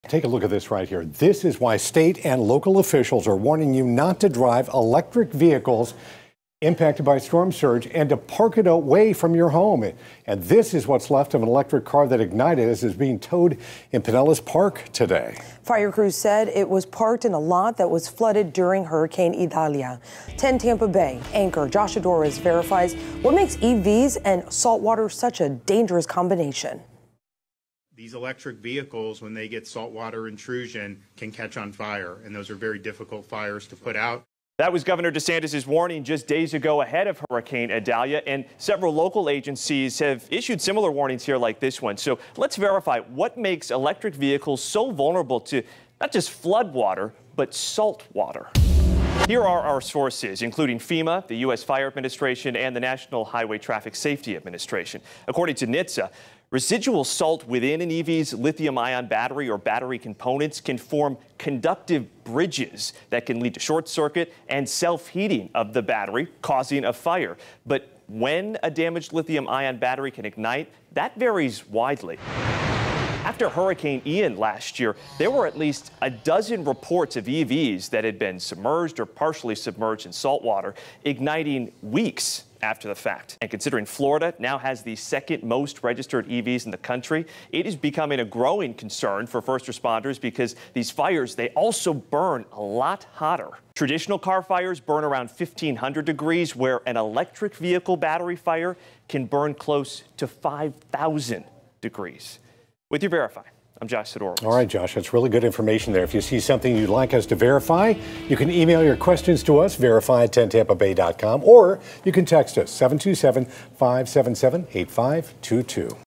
Yes, electric vehicles are prone to igniting in flood, saltwater
A day after being partially flooded by saltwater from storm surge caused by Hurricane Idalia, a Tesla being hauled away on a flatbed ignited in flames.

Fire crews who responded to the parking lot on 66th Street N and Park Boulevard said it took nearly an hour to extinguish the fire – something they said is typical with electric vehicles.

In the days leading up to Hurricane Idalia making landfall in Florida, several state and local officials issued warnings about electric vehicles and storm surge, including Florida's CFO Jimmy Patronis.